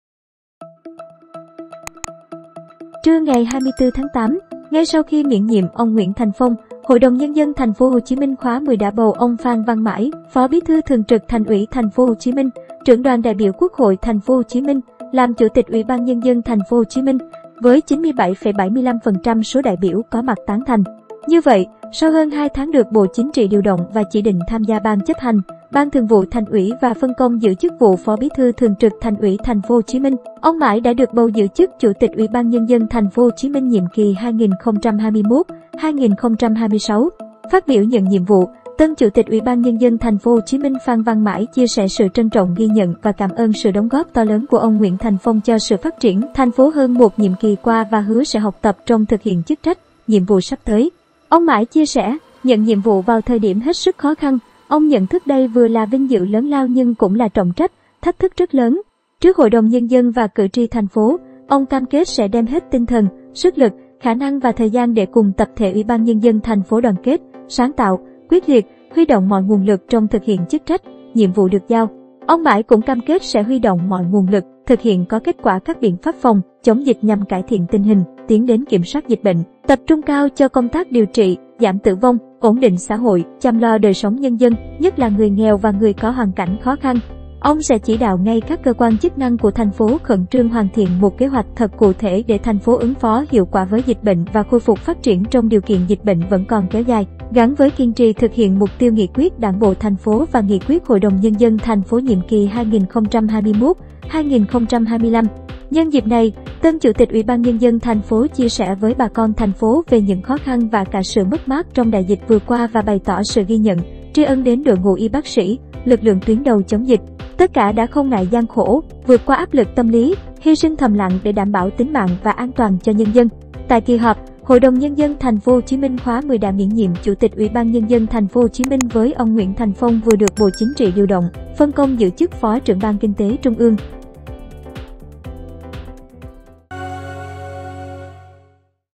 Tân Chủ tịch TP.HCM Phan Văn Mãi: Tập trung cải thiện tình hình, tiến đến kiểm soát dịch COVID-19
Ông Phan Văn Mãi - phó bí thư thường trực Thành ủy, trưởng Đoàn đại biểu Quốc hội TP.HCM - chính thức được HĐND TP.HCM khóa X bầu giữ chức Chủ tịch UBND TP.HCM thay cho ông Nguyễn Thành Phong.